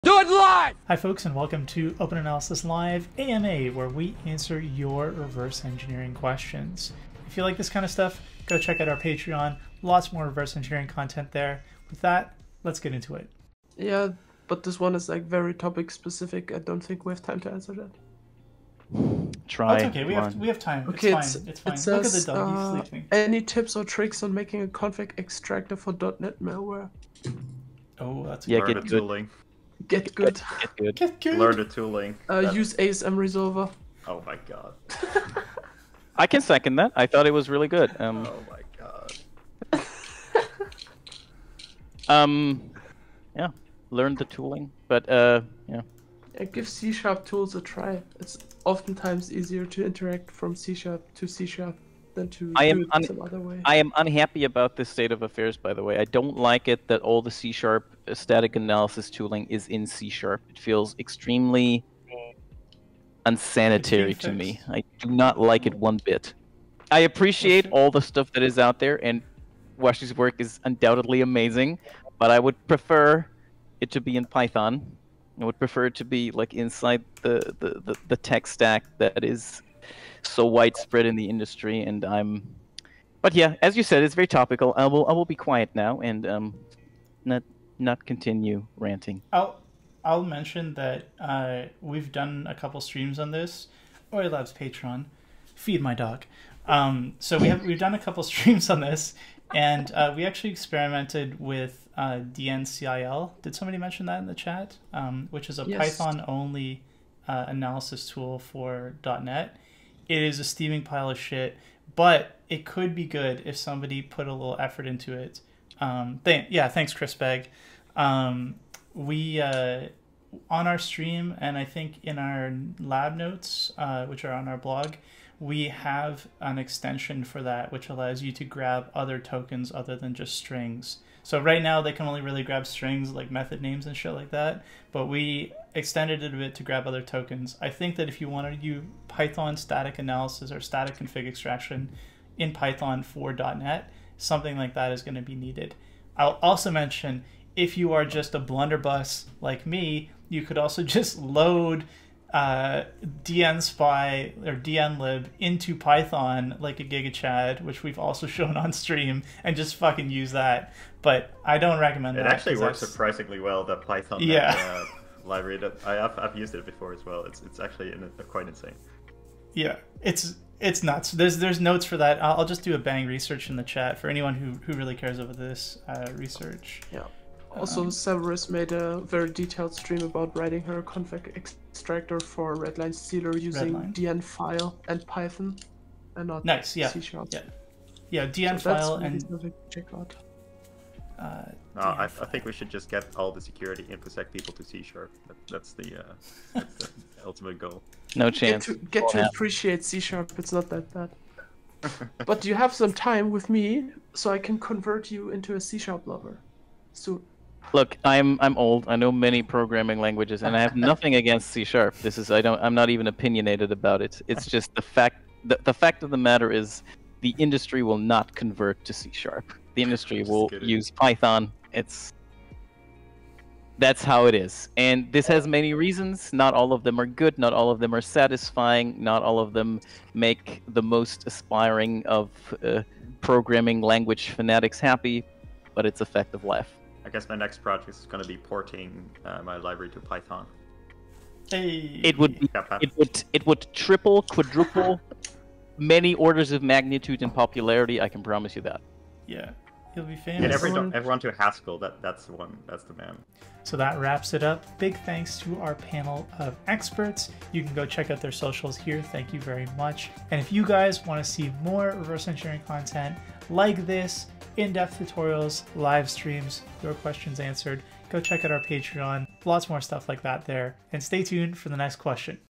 Tips For Writing a .NET Static Config Extractor for Malware [ Reverse Engineering AMA ]
What are some tips for dealing with static config extraction of .NET malware?

Big thanks to all the reverse engineers who helped us put this together!